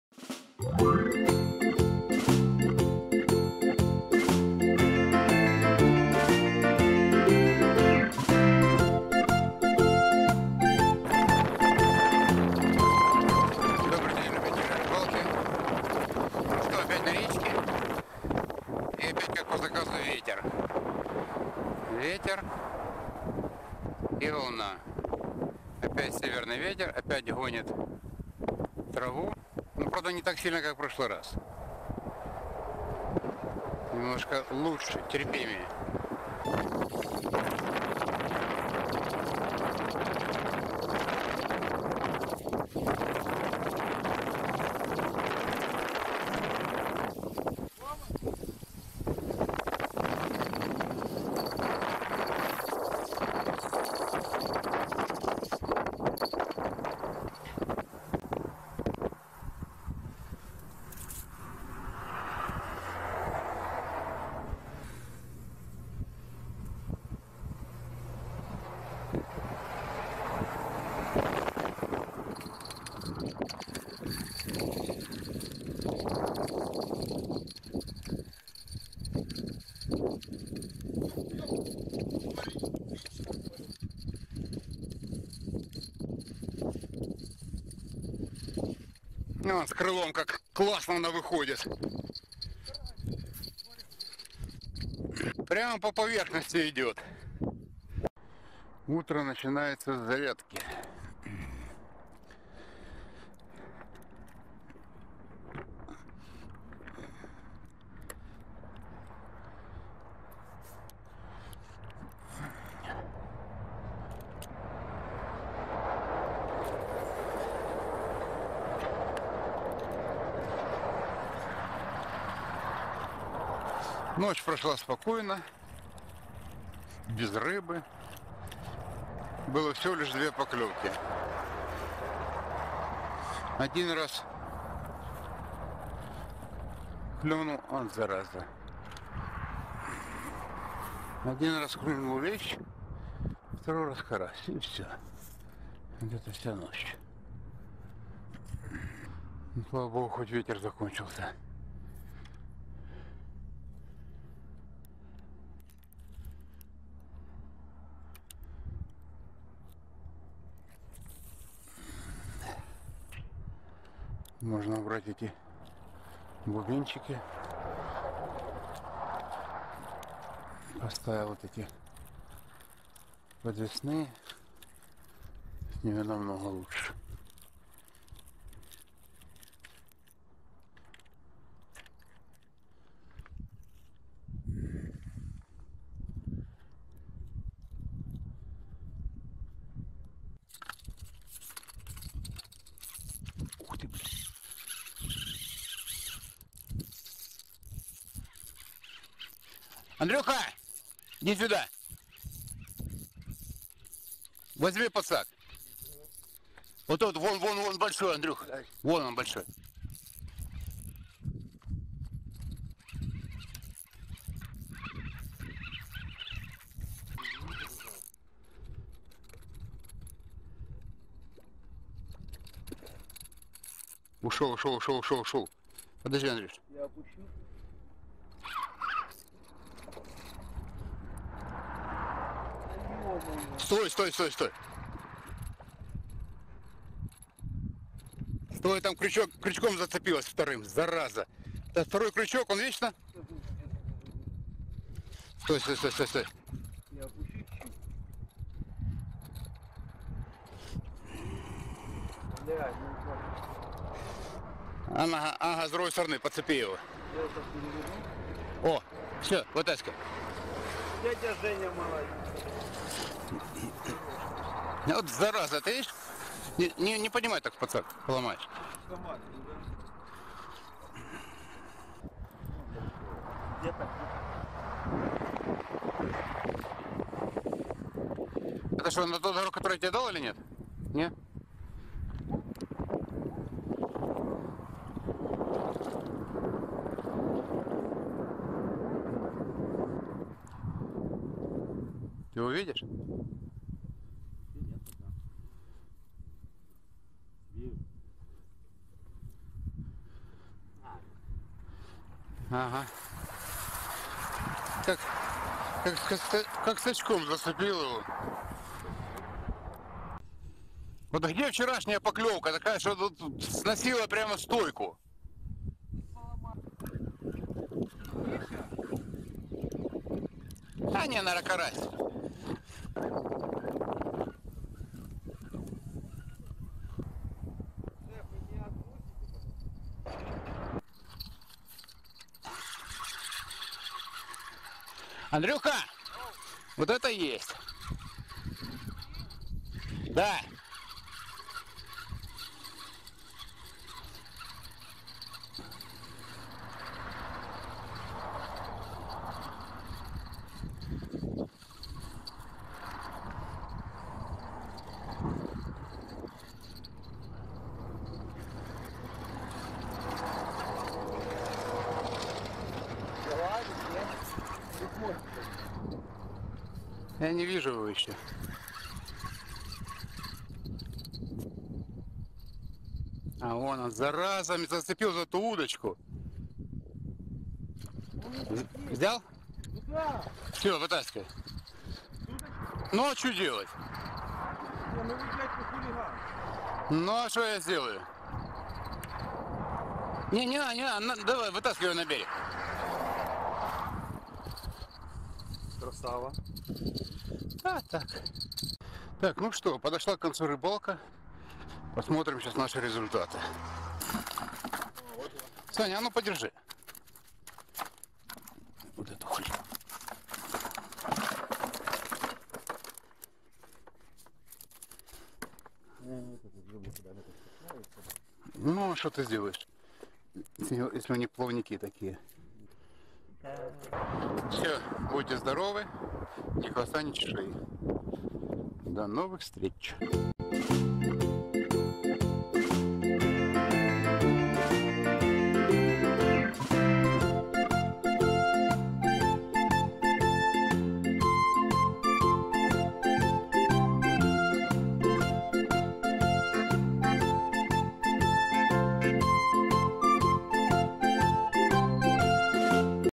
У детей снова школа , а у нас новая рыбалка
Рыбалка как и все на Земле циклична ,Опять ветер, опять трава.  Но нас этим не испугать. Кроме самой рыбалки показал работу крыла